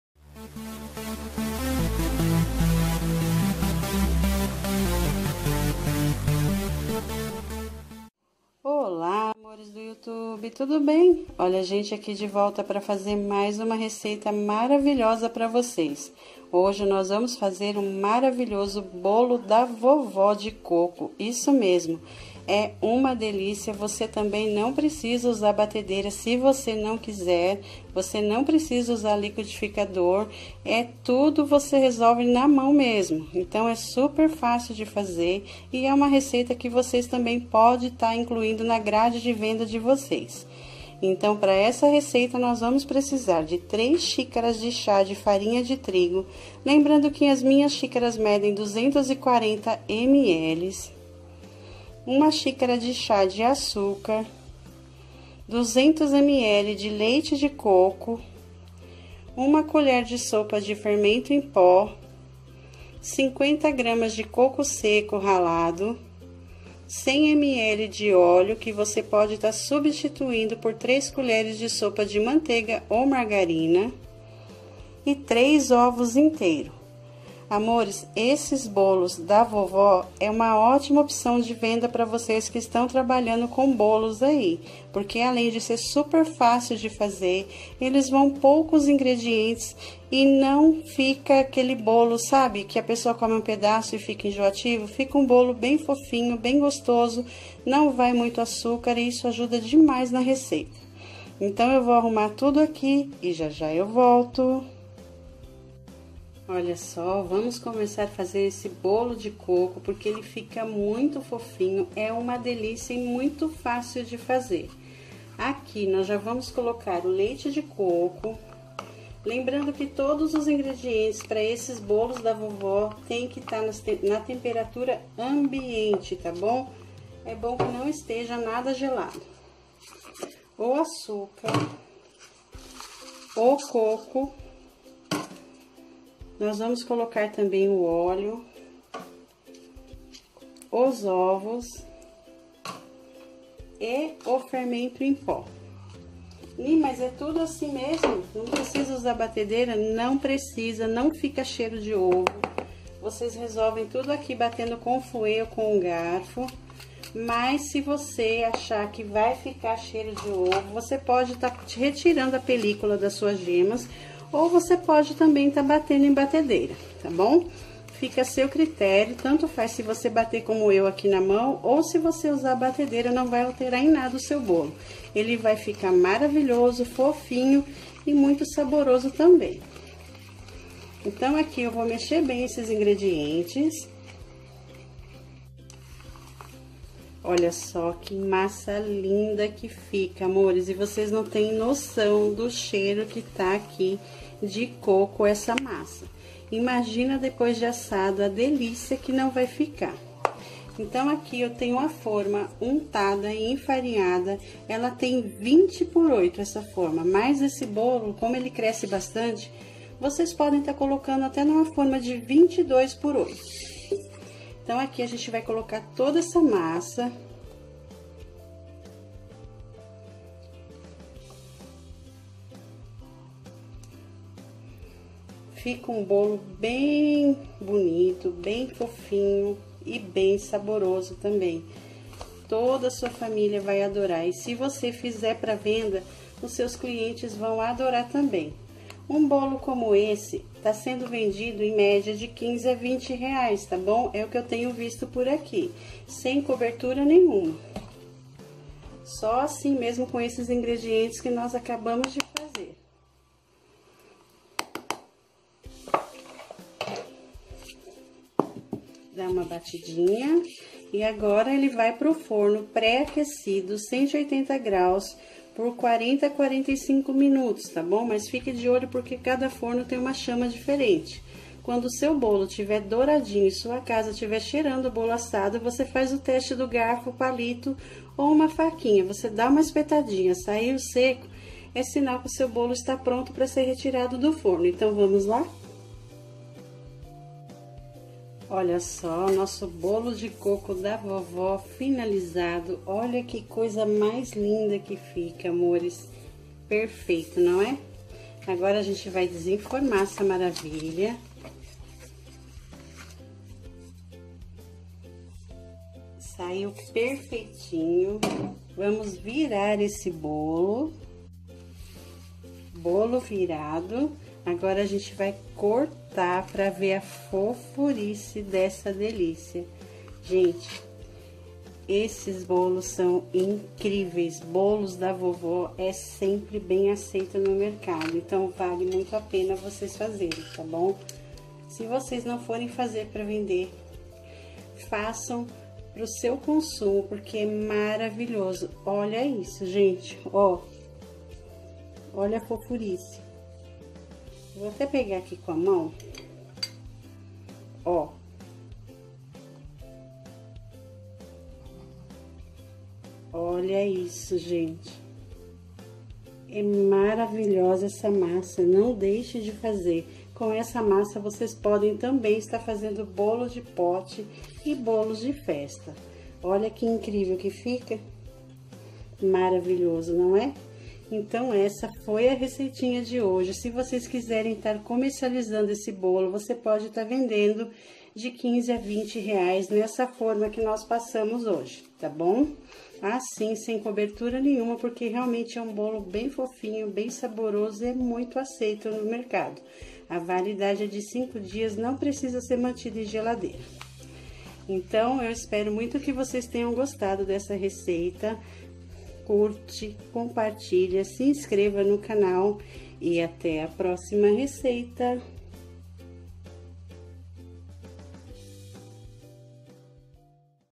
BOLO DE COCO FOFINHO E FÁCIL DA VOVÓ .
Obrigada a todos pelo carinho beijos 💋💋

Ingredientes ⬇️
Três xícaras de chá de farinha de trigo ✔️
Uma xícara de chá de açúcar ✔️
200ml de leite de coco ✔️
50g de coco seco ralado ✔️
Três ovos inteiros ✔️
Uma colher de sopa de fermento em pó ✔️
100ml de óleo,ou três Colheres de sopa de manteiga ou margarina ✔️
Validade é de 5 dias ✔️
Sugestões de preços 💲
15 a 20 reais ✔️
Lembrando que são apenas sugestões OK
Modo de fazer ✔️ Vamos ver o vídeo ❤️😘

ENDEREÇO POSTAL ⬇️
Onicia Alves
Caixa postal N°13
Itaquaquecetuba-SP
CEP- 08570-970

Que Deus abençoe a todos pelo carinho ❤️💋✔️